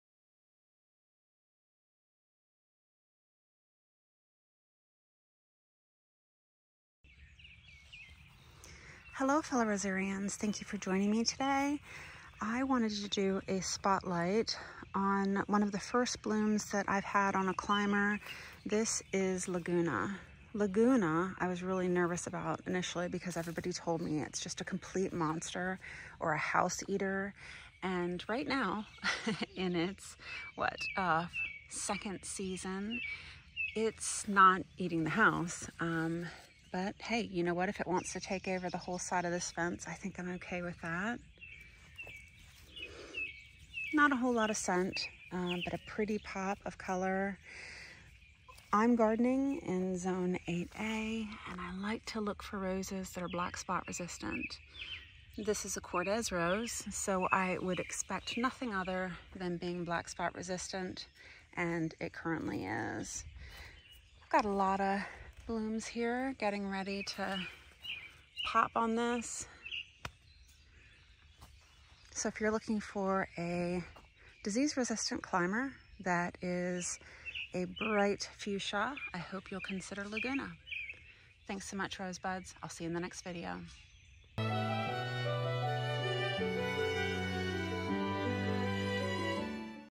🌹Arborose® Laguna™ Climbing Rose / KORadigel/ Zone 8a
A short video of our Laguna rose, one of our many beauties from Kordes.

🌹 SOCIAL MEDIA 🌹
🌎 Rose Geeks Facebook Group to Share Your Roses